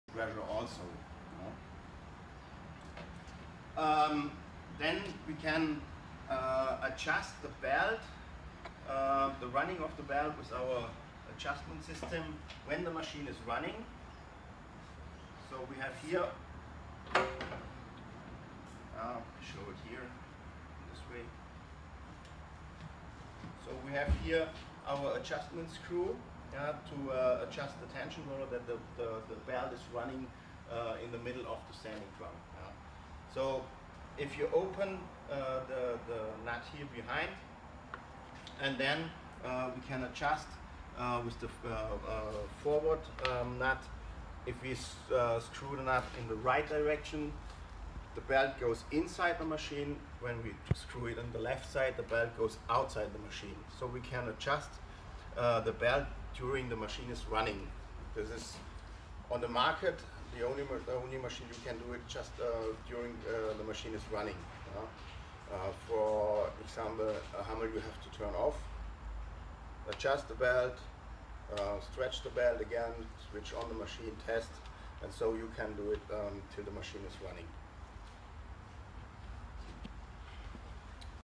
Frank Cobra floor sander Belt adjustment
A demonstration of the Frank Cobra Belt Sander. Adjusting the belt , Demonstrated by Klaus Thiele from Frank - Pallman Germany.